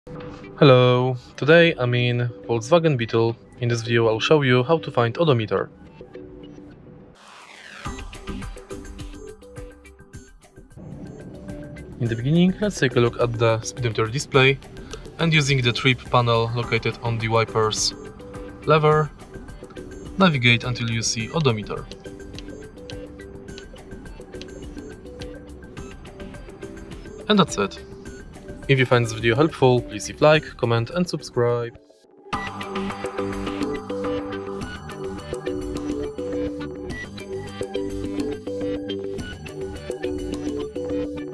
How to Find Odometer on Speedometer Display in Volkswagen Beetle ( 2011 – 2017 ) | Find Mileage
Find out more info about Volkswagen Beetle ( 2011 – 2017 ) :
Would You like to know how to find the Mileage of Your Volkswagen Beetle? Thanks to Odometer You'll be able to check how many kilometers at all Your car has made. Follow the video above to learn how to better manage Your Volkswagen Beetle!

How to Find Odometer? How to Find Mileage? How to Check Mileage?

Years of Production:

2011, 2012, 2013, 2014, 2015, 2016, 2017

Generation:

I Gen

Series:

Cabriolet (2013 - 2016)
Hatchback 3d FL (2017)